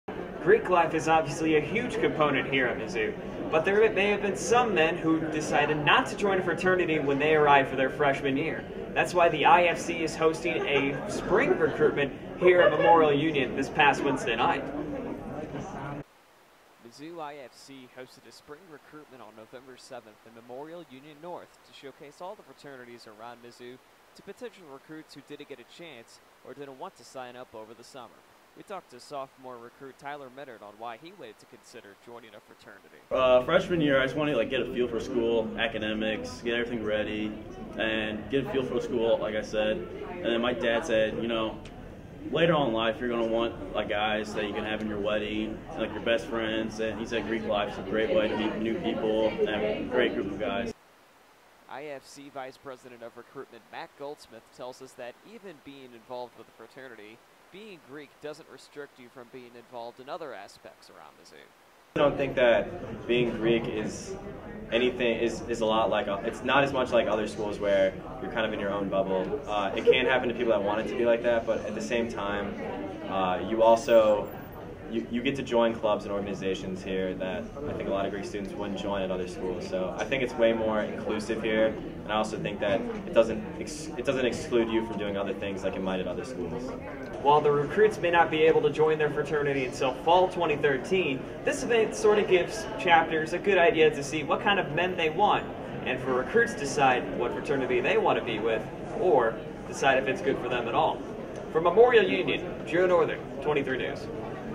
IFC Spring Recruitment 2012
On November 7th the Inter Fraternity Council, otherwise known as the IFC, held its very first open house to give young men the chance to get involved in spring recruitment. Most of the 28 chapters at Mizzou set up tables in the second story of Memorial Union so visitors could walk by and talk with each fraternity one-on-one.
Fraternities came equipped with posters, paraphernalia, and even paddles to show off to the possible incomers. This spring recruitment will give the men that were unable to attend fall recruitment, and people who were unsure but now want to join a second chance to get involved with the Greek community. Recruitment normally takes place during the latter weeks of summer. This prohibits some from traveling to get involved.  The Vice President of Recruitment, Matt Goldsmith, explained why Greek life is a big part of modern Mizzou, because of its extensive history. This event was planned by Jon Strope, the Vice President of Public Relations, and Goldsmith. The IFC is hoping to broaden their membership by providing an additional recruitment every spring.